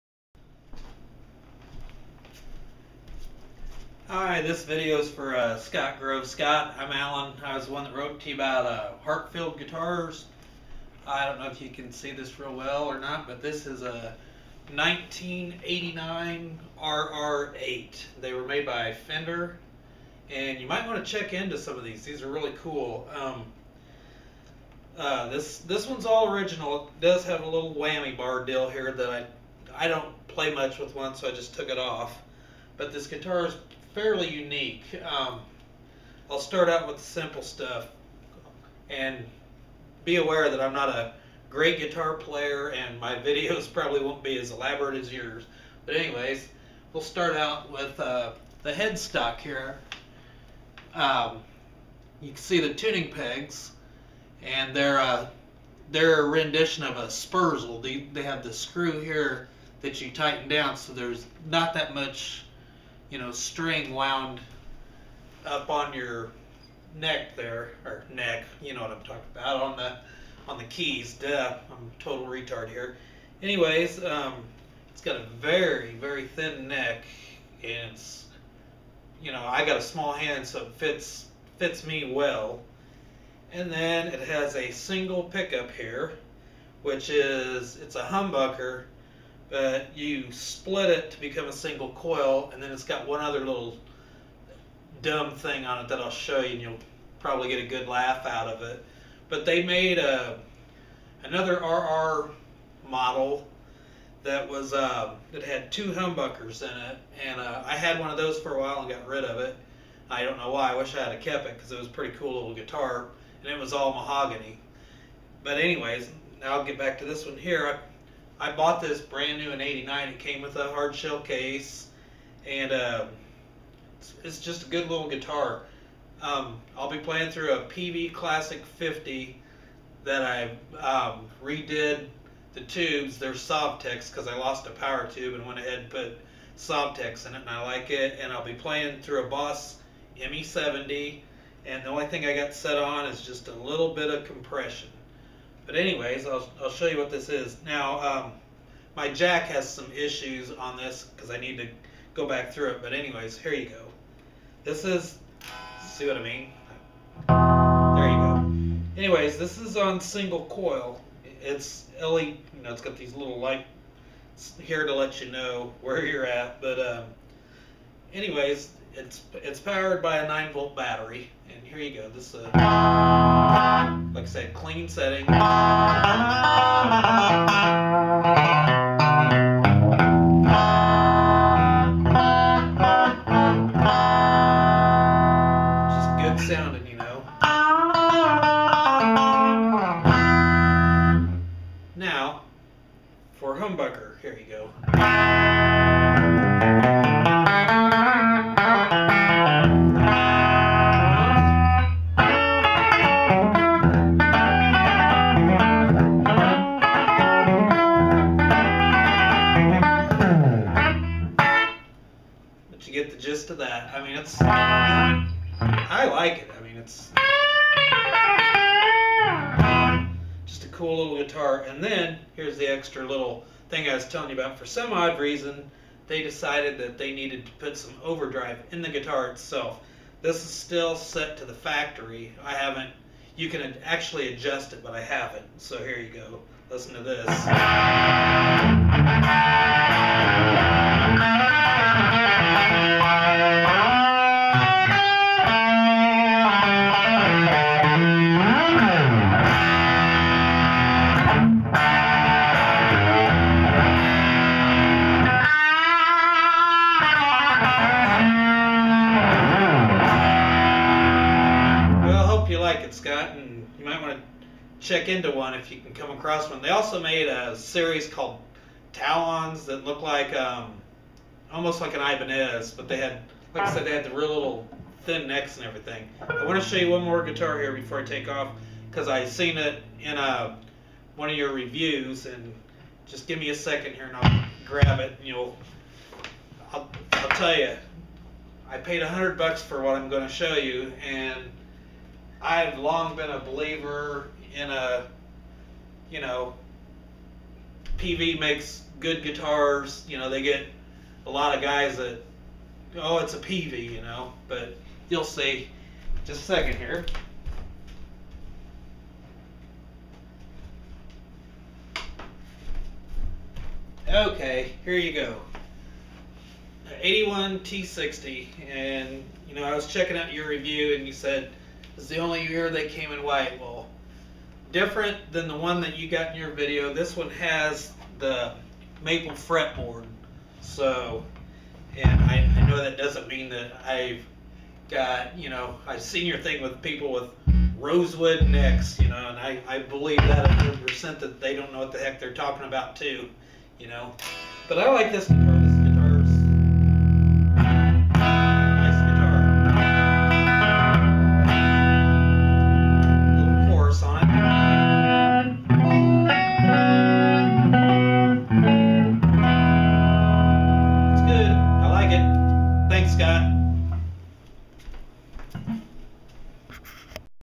review for scott grove
this is my 1989 heartfield rr8.hoping scott grove will check into them.